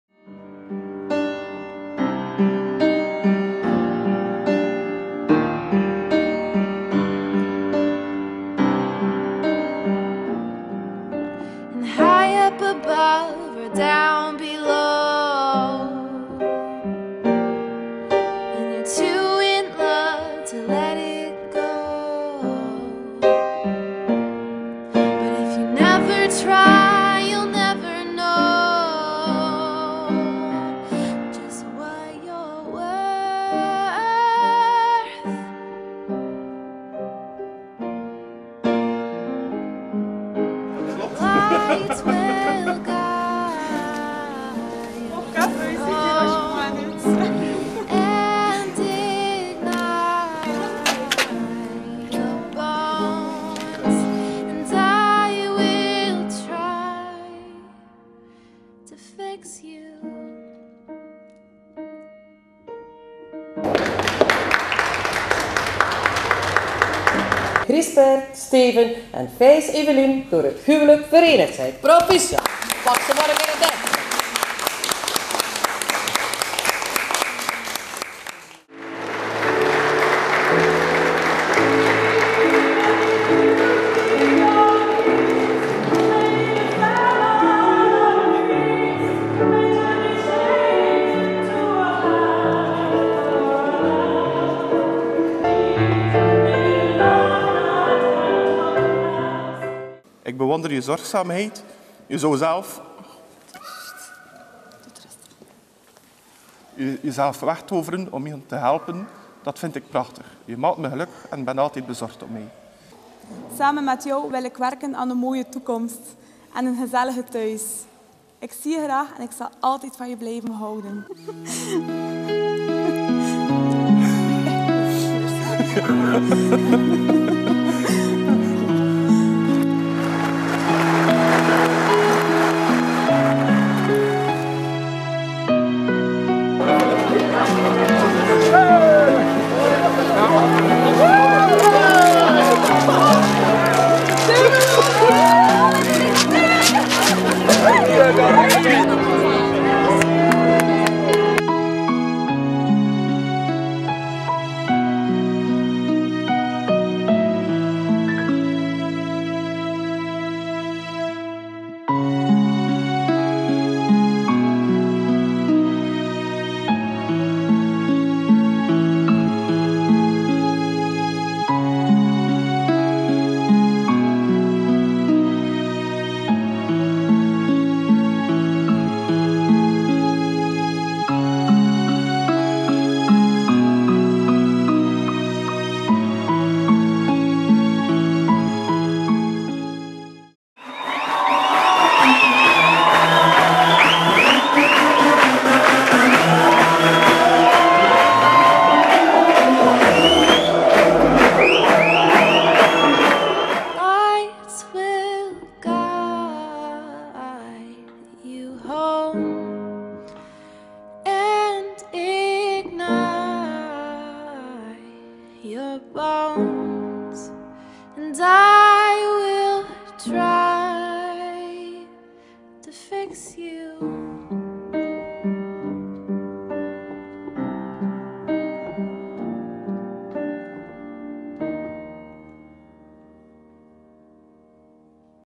Evelyn en Steven
Evelyn en Steven - highlights van hun huwelijk